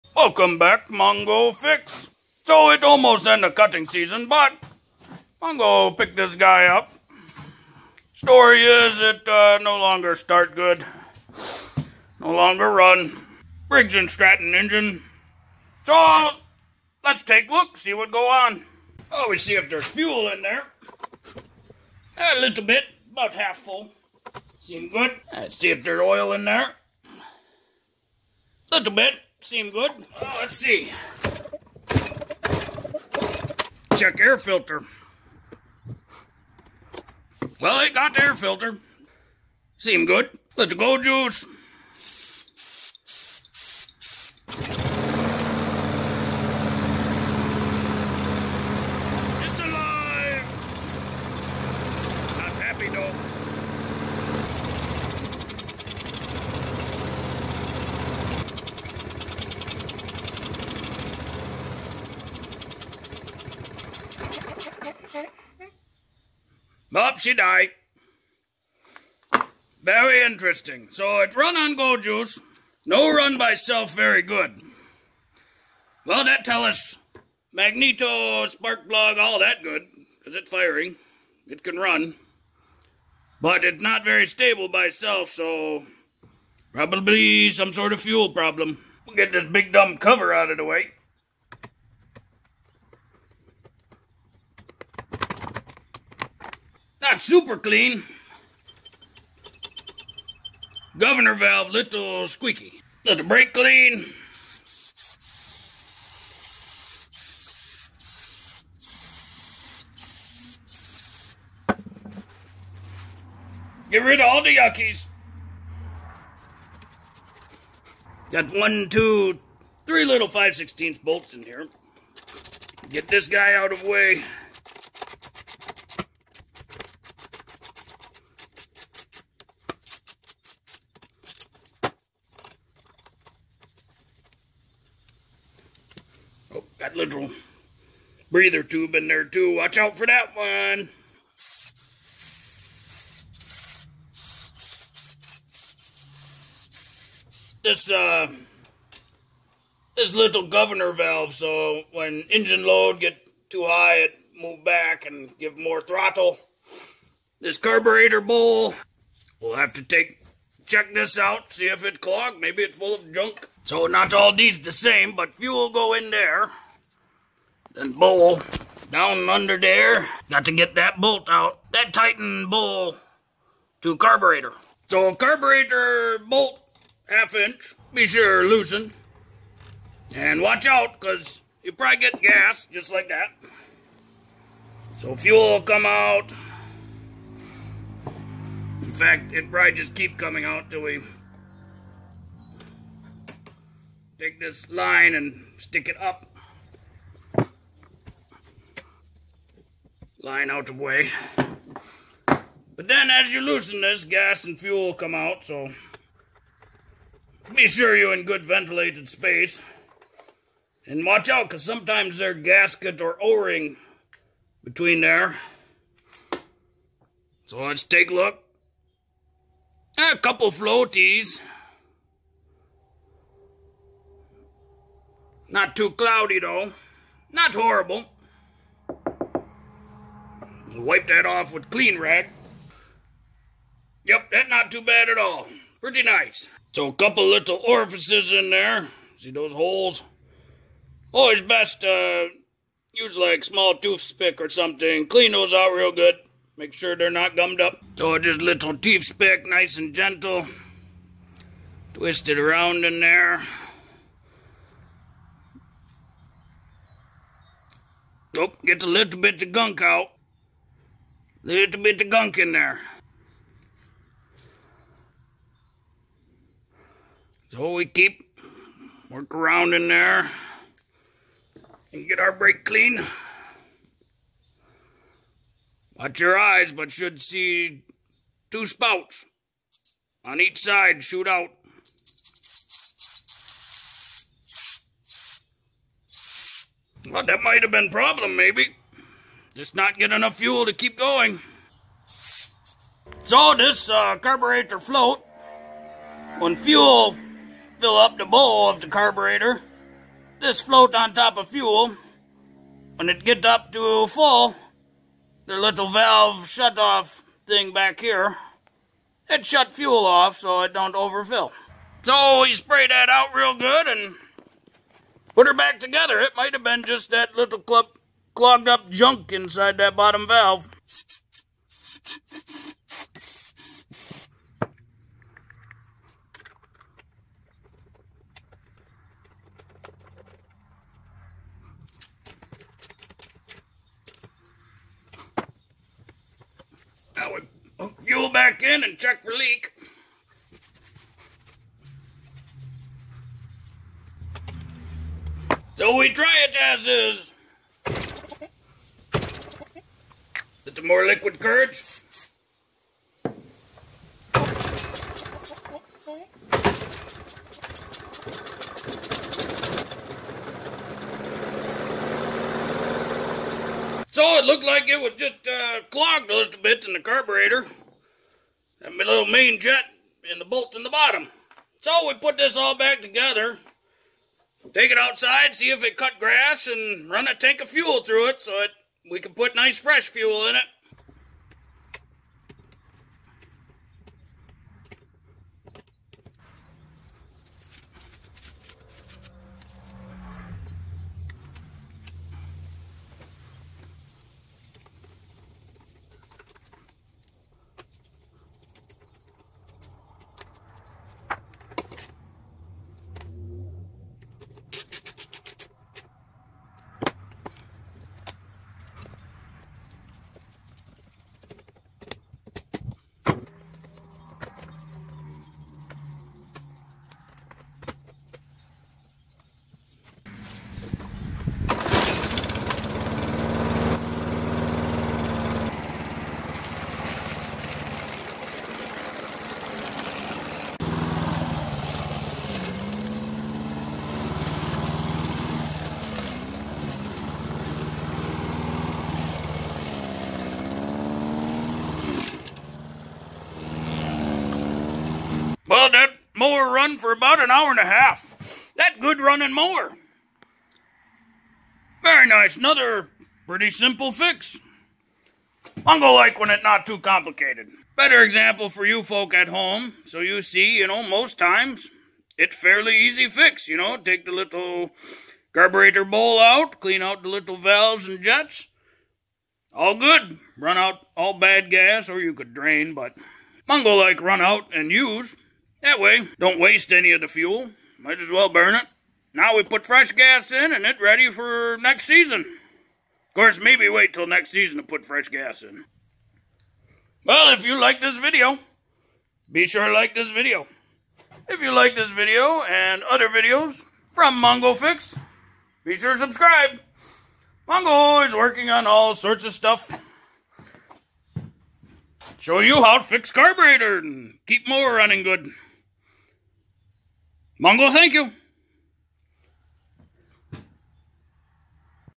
Troy-Bilt Push Mower fuel issues
Mongo pick up this Troy-Bilt push mower and it clam to have no start issue.
so check the basics - oil, fuel and filter. Give a little go juice (starting fluid) and it fires up but not stay running. Like saw in last video if engine run on go juice then but not by self then coils and spark are good but something wrong with fuel system. So we take a look at the fuel system and see what's going on.
Be sure you in well ventilated space when opening fuel system, fumes are dangerous (fire and health risks)
Look like fuel in bowel have some floating material and some cloudy material this can be sign of bad fuel. This material can cause clogs in the small orifices in the carburetor. But Mongo seen WAY worse so we clean out. Check the main jet. This one the main jet part of the bottom bolt so we use small pick to push out any hard material and we get some out. This material can cause fueling issue to carb which can cause the issue we are seeing. (not getting enough fuel to keep engine running on its own) Good to use brake clean to blast any remaining debris out of the jets. Now put back together and see how it works.

00:00 HIs STORY
00:48 GO juice test
01:29 Remove cover
02:37 Remove carburetor bowel
03:56 Main jet clean out
04:53 Basic Carburetor walkthrough
05:25 Reinstall
07:01 Test and run out of fuel

Mower runs great now!
Hopefully this video help you if you have mower that doesn't want to start or stay running.

If you like this video, be sure to like this video.

MONGO THANK YOU!!!